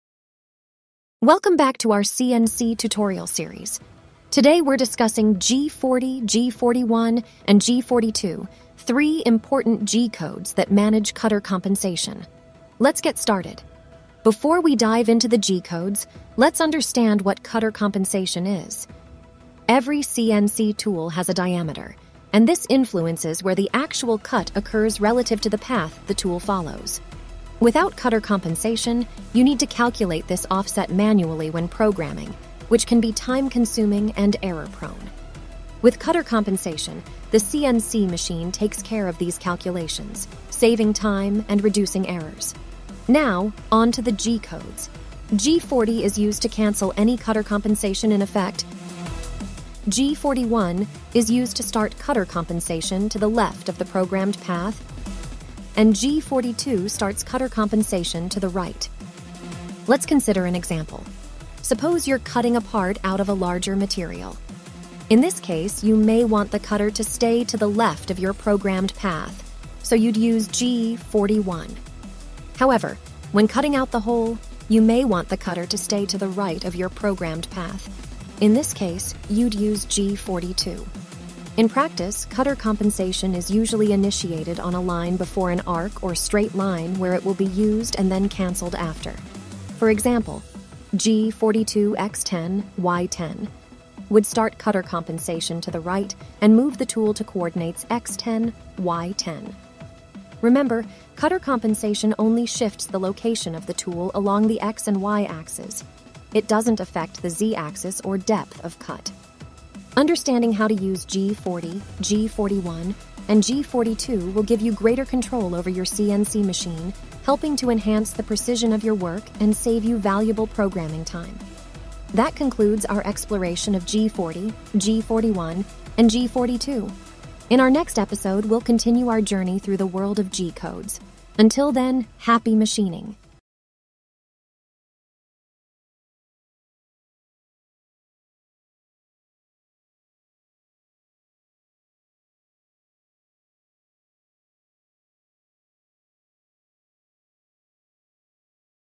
Part 8/15 | Optimize CNC Cutting with Cutter Compensation: Mastering G-Codes G40, G41, and G42
Welcome back to our CNC tutorial series! In this episode, we focus on G40, G41, and G42—key G-codes for managing cutter compensation. Learn how to enhance the precision of your CNC work by allowing the machine to automatically adjust for tool diameter.

🔹 What You'll Discover:

📌 The importance of cutter compensation in achieving precise cuts
📌 How G40 cancels cutter compensation, while G41 and G42 initiate it to the left and right of the programmed path, respectively
📌 Practical examples of when and how to use these cutter compensation G-codes

to continue your CNC learning journey!